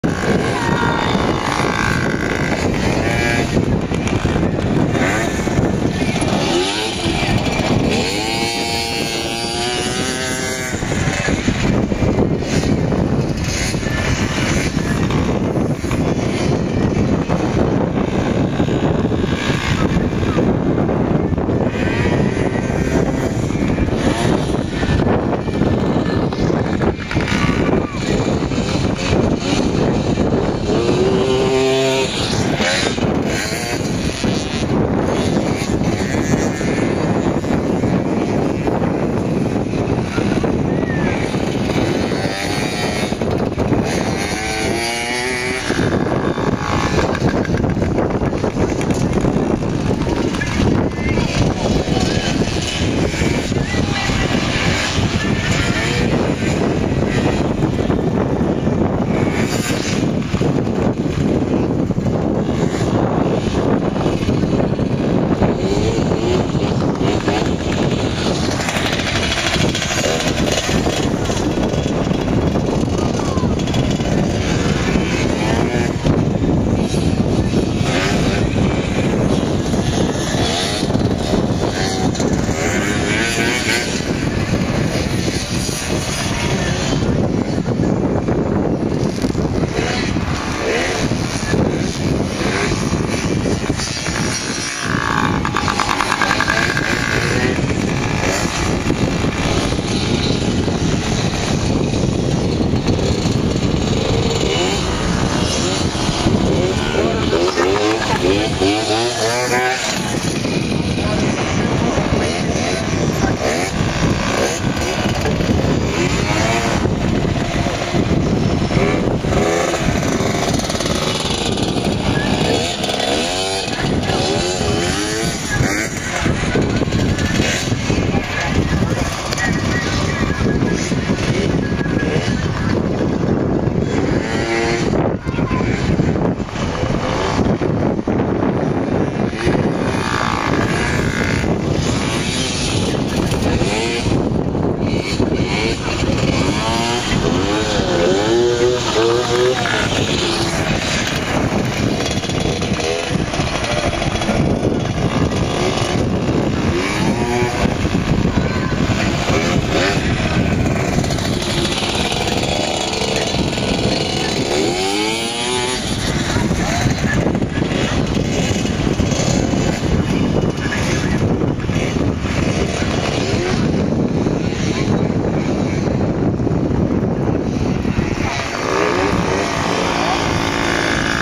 marshall byford first time at motocross bonville farm essex pw50
my son marshalls  first time at motocross on his pw50, aged 7..